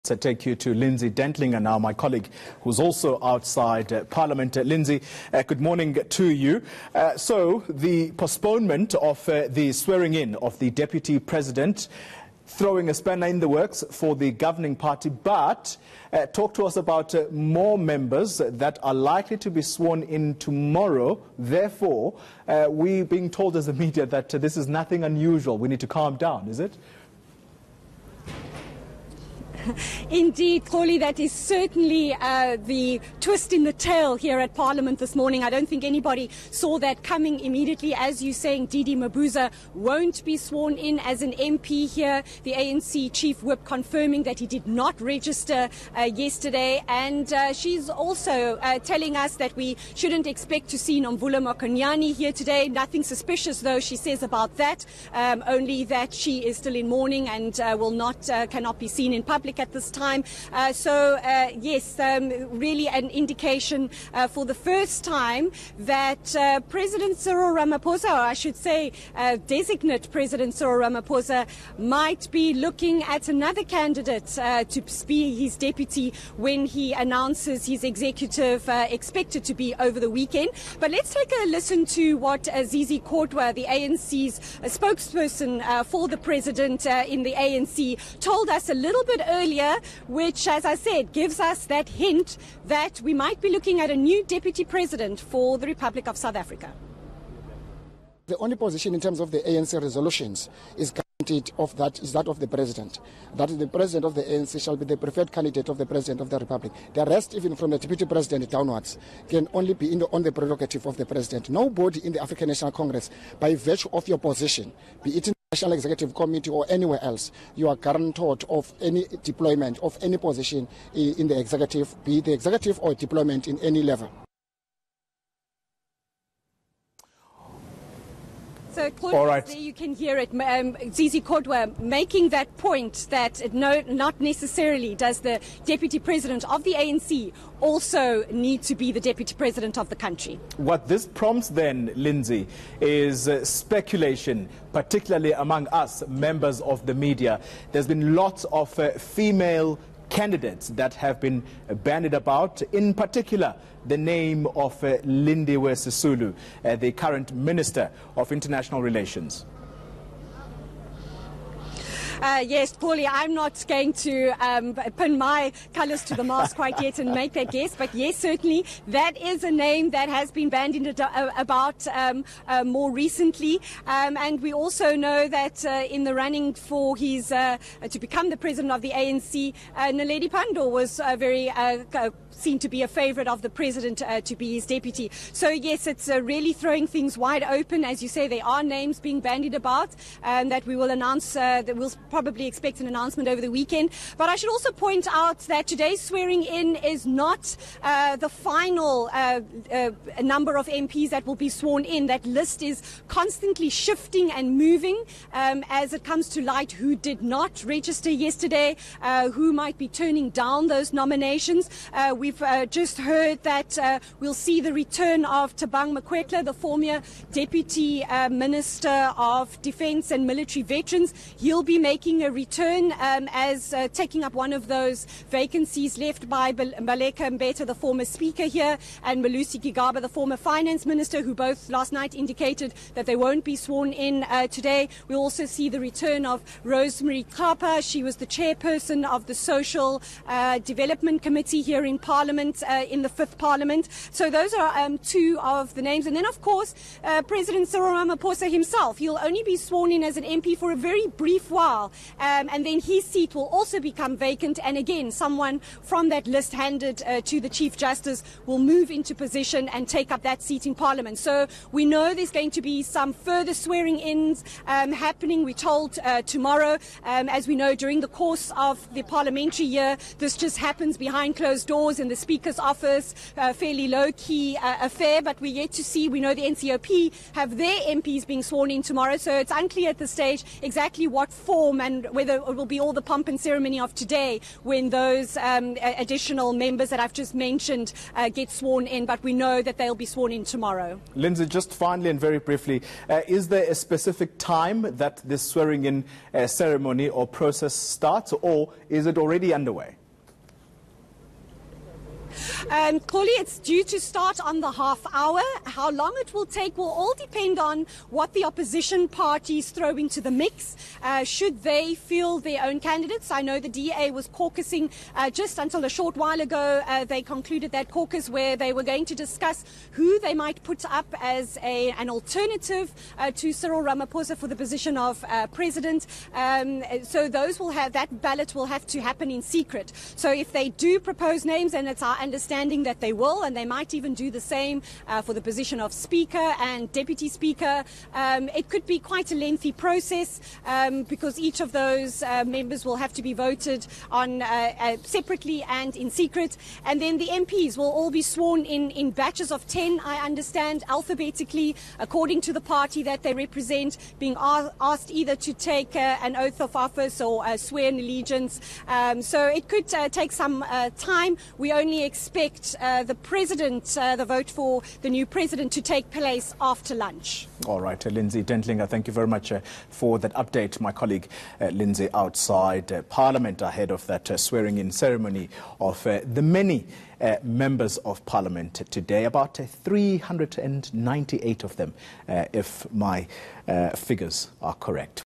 ANC member Nomvula Mokonyane swearing-in delayed
David Mabuza is not the only ANC member to delay swearing-in as a member of parliament on Wednesday. Courtesy of DSTV403